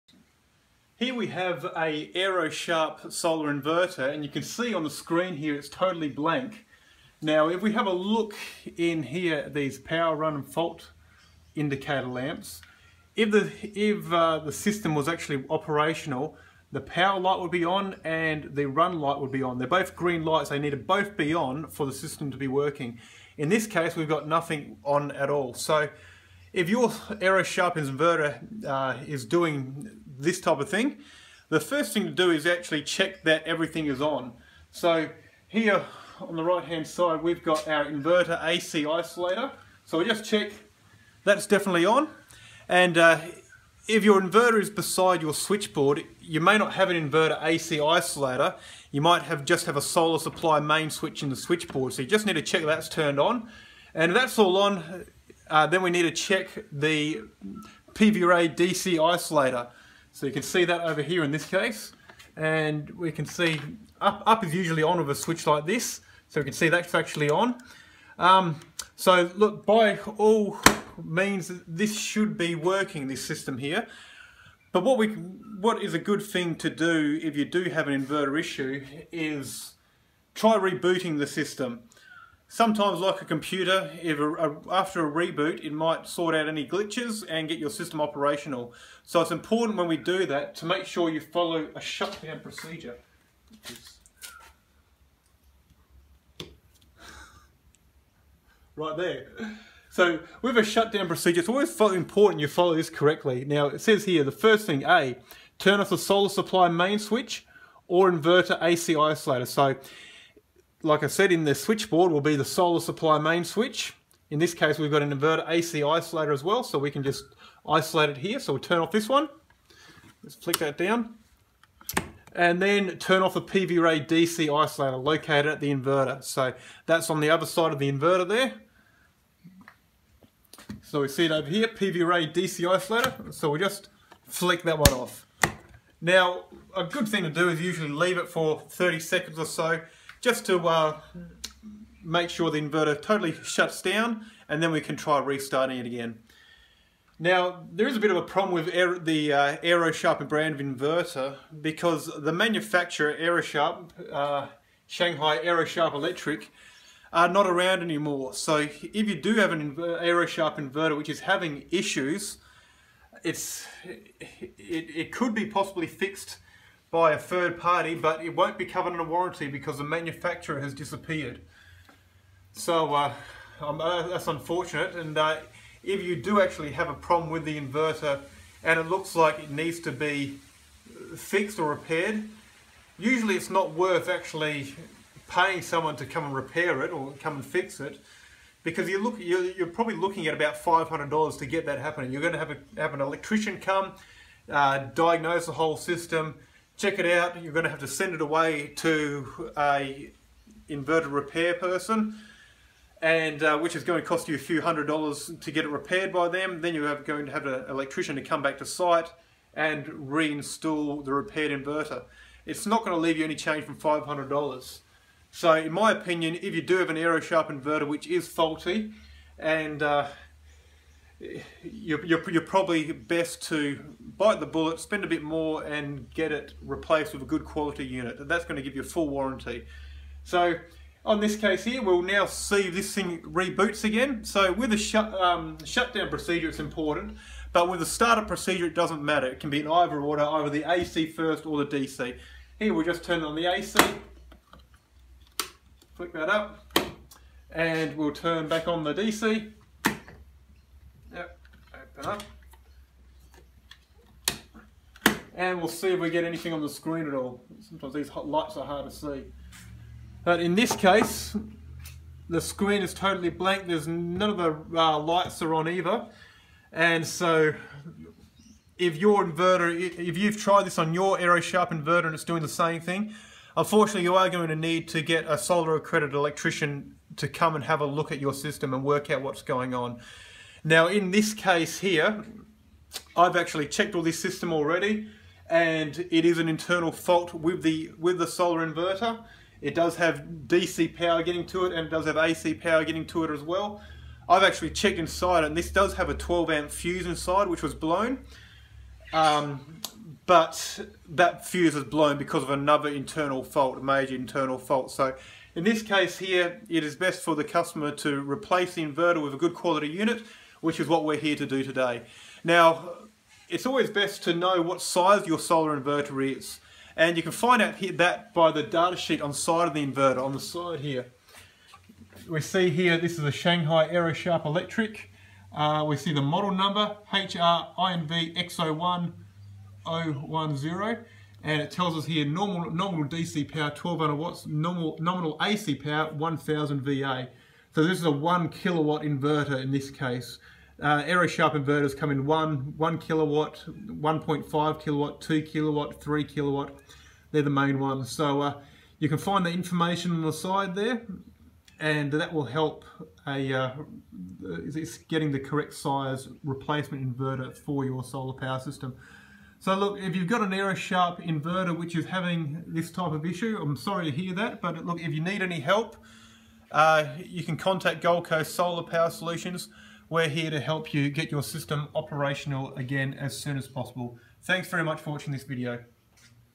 Aero-Sharp Inverter Blank Screen
Is the screen on your Aero-Sharp inverter completely blank? This could be due to damage from the sun (if it's installed outside) or it may be that the inverter isn't working at all. Find out what you can do about it here with Gold Coast Solar Power Solutions.